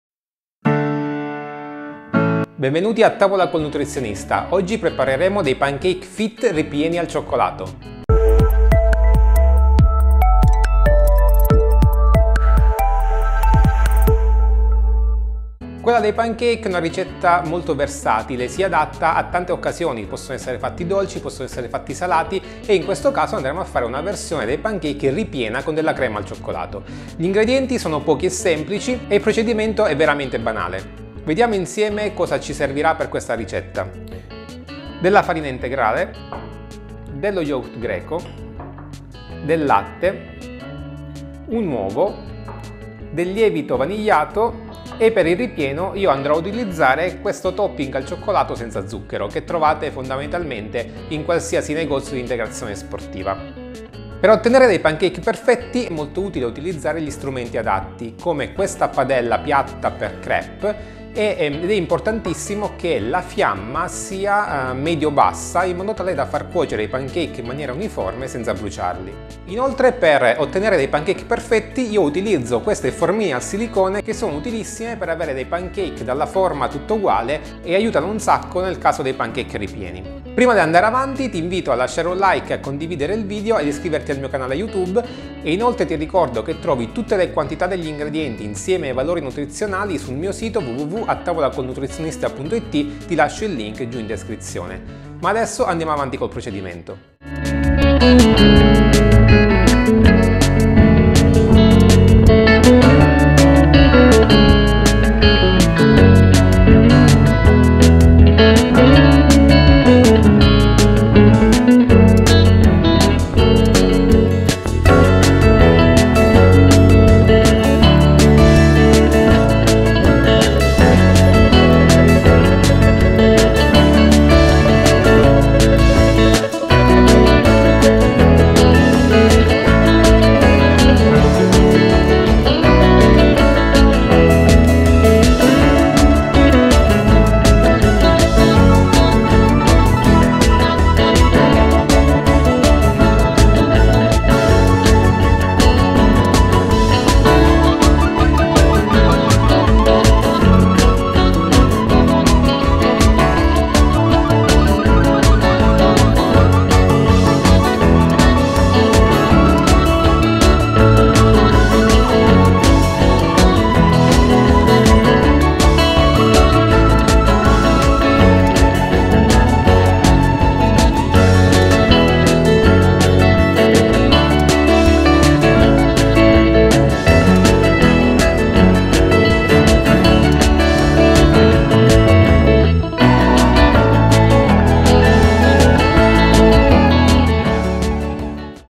PANCAKE RIPIENI al CICCOLATO - Ricetta Fit
Quella dei pancake light integrali è una ricetta facile e sfiziosa. Servono pochi ingredienti e il risultato è garantito! Oggi te li propongo in una sfiziosissima versione ripiena al cioccolato!

► Padella per Pancake
► Formine per Pancake
► Piastra ad induzione
► Porta Posate

🎥 La mia videocamera
🔭 Il mio obiettivo
🎤 Il mio microfono
♟ Il mio cavalletto
💡 Le mie luci